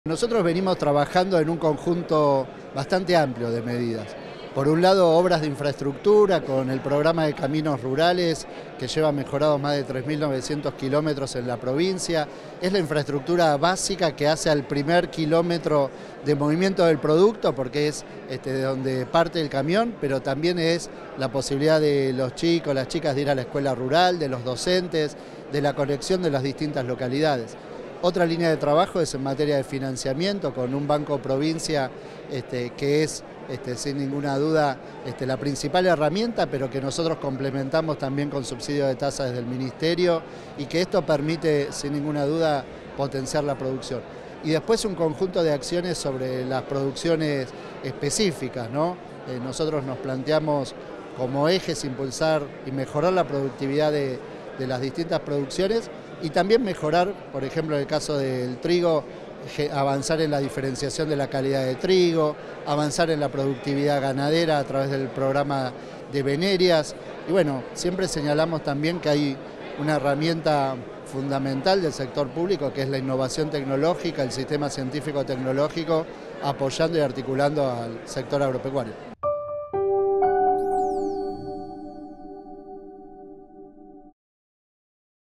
El ministro de Desarrollo Agrario bonaerense Javier Rodríguez partcipó del Congreso Maizar
El ministro de Desarrollo Agrario bonaerense, Javier Rodríguez, formó parte del Congreso Maizar, que pertenece a la Asociación Maíz y Sorgo Argentino, para brindar una charla centrada en las políticas de Estado potencialmente aplicables a la bioeconomía.